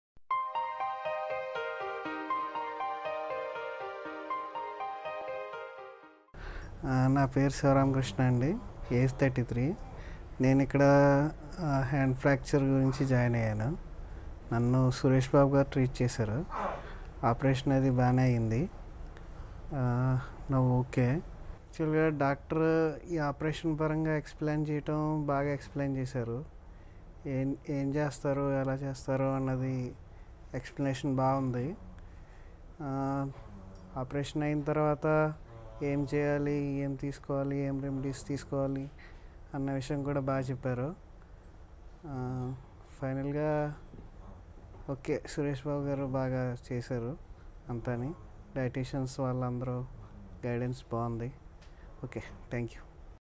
Mr. P. Siva Rama Krishna's Testimonial :: Metacarpal Fracture :: Dr. Suresh Babu.CR
Mr. P. Siva Rama Krishna, 33 years old from Hyderabad, was admitted
for 4th and 5th Metacarpal Fracture and was successfully treated at KIMS Hospitals by Dr. Suresh Babu.CR, MS (Ortho),Fellow in Joint Replacement